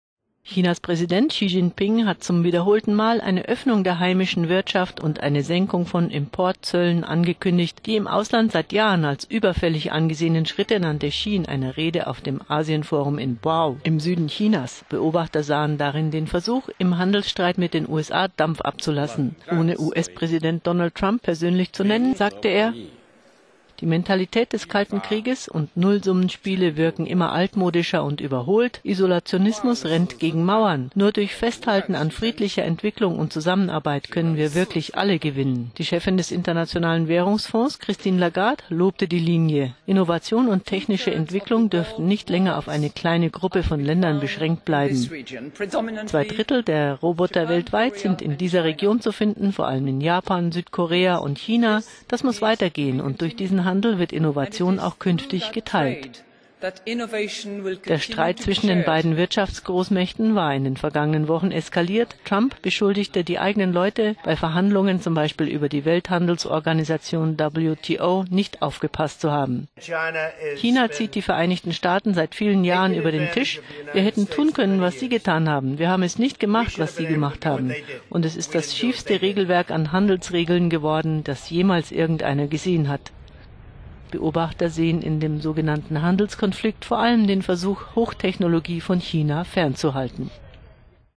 Handelskonflikt mit den USA: China hält den Ball flach
Chinas Präsident Xi Jinping hat zum wiederholten Mal eine Öffnung der heimischen Wirtschaft und eine Senkung von Importzöllen angekündigt…